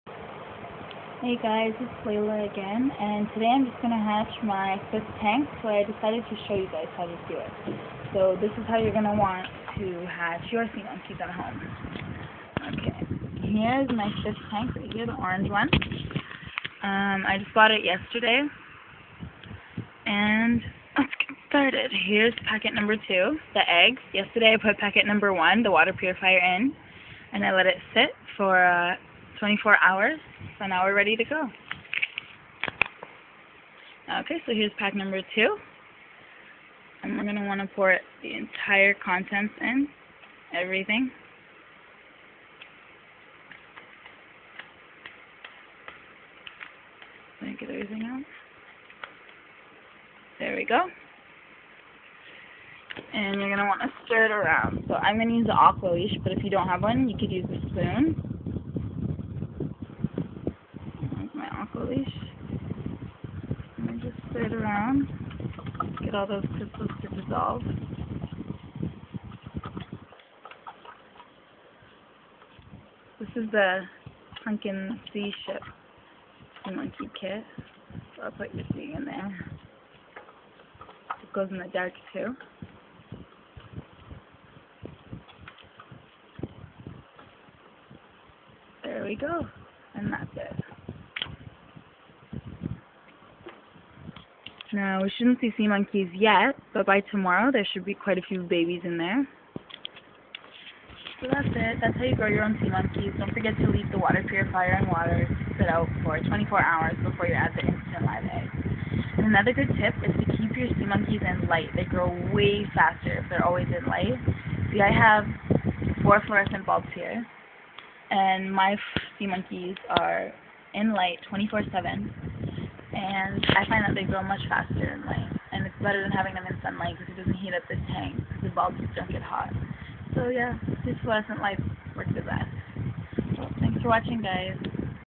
How to Hatch sea monkey eggs.
Leila shows How to Hatch sea monkey eggs.